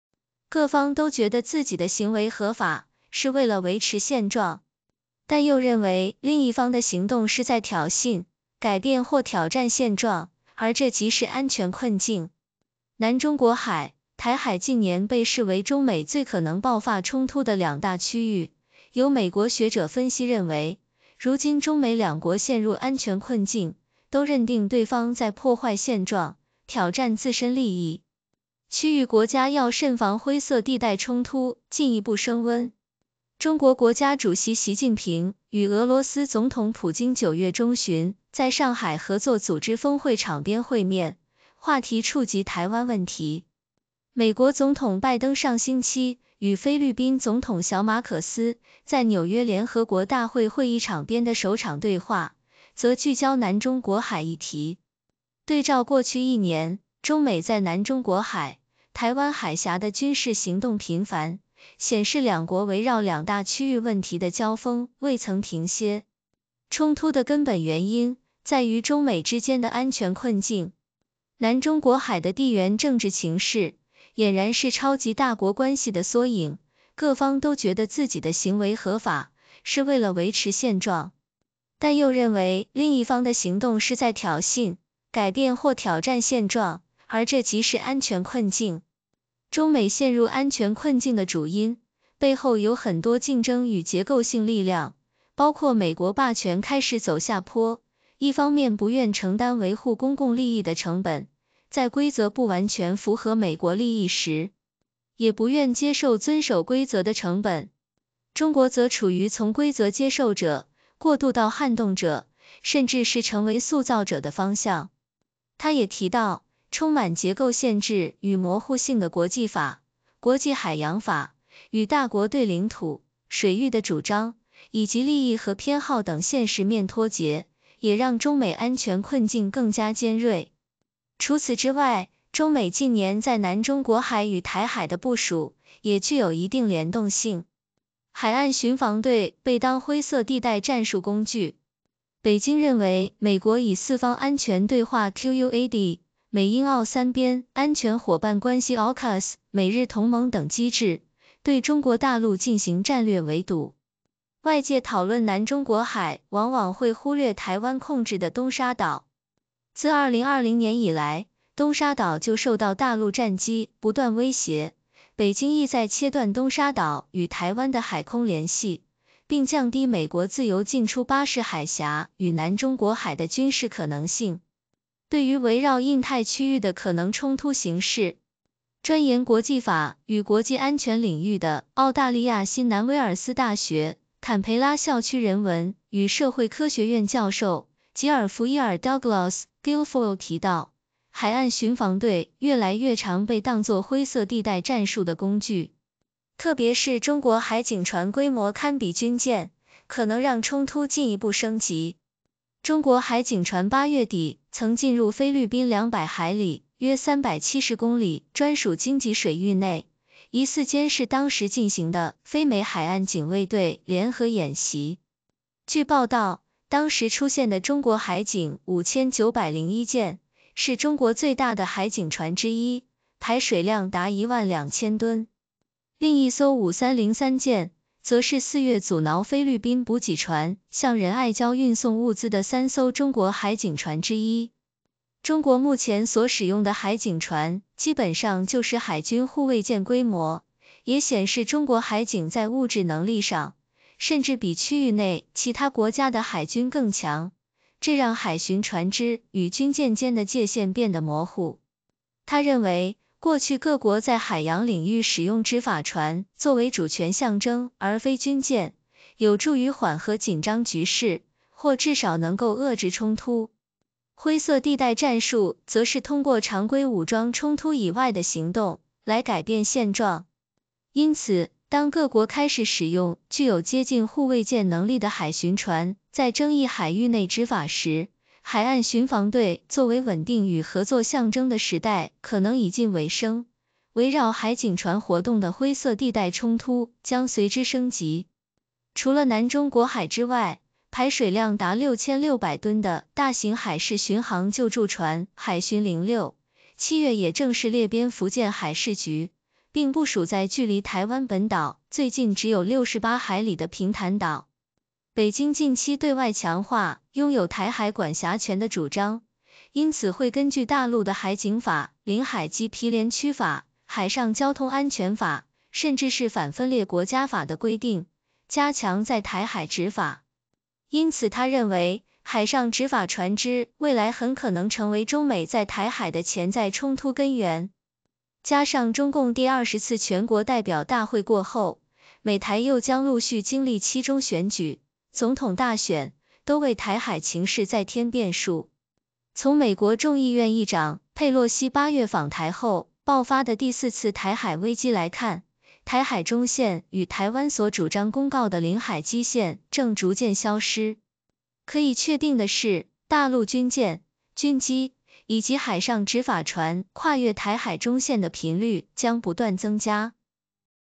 中美陷安全困境灰色地带冲突或升级
各方都觉得自己的行为合法，是为了维持现状；但又认为另一方的行动是在挑衅、改变或挑战现状，而这即是“安全困境”。